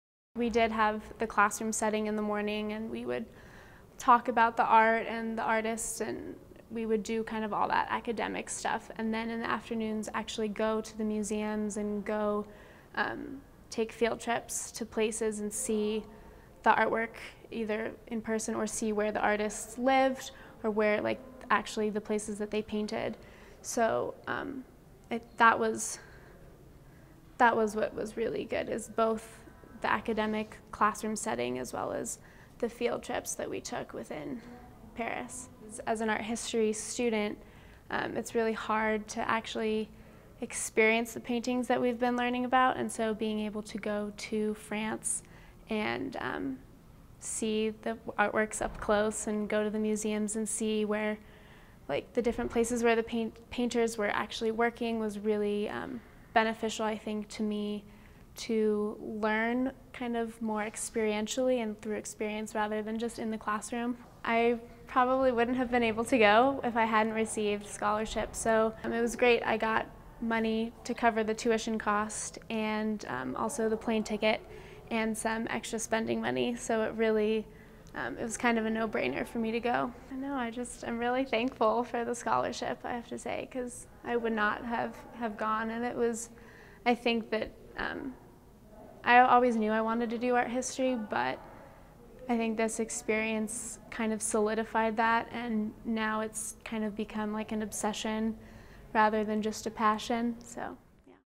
Art and History In Context: Tinka Avramov '15
Student Tinka Avramov talks about her experience taking an art history class in Paris.